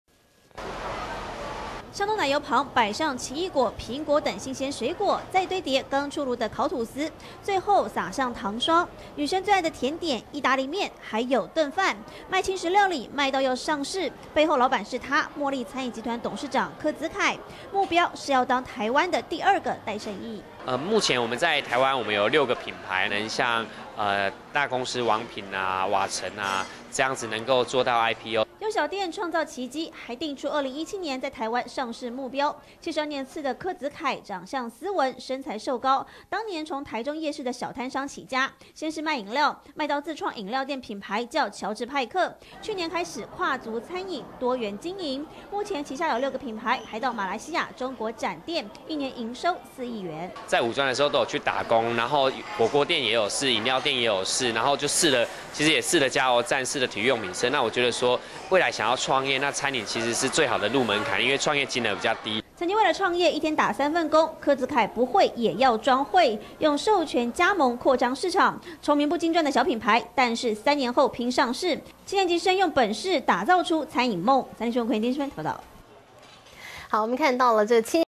七年級老闆柯梓凱 開餐廳拚上市│三立財經台
七年級生賣飲料，能賣到變成餐飲集團，還要上市！他是墨力餐飲集團董事長柯梓凱，當初從台中夜市的小攤商起家，靠著授權，加盟擴張市場，進軍海外，如今手上有六個品牌，一年營收4億元，吸引外資投資，預計3年後拼上市!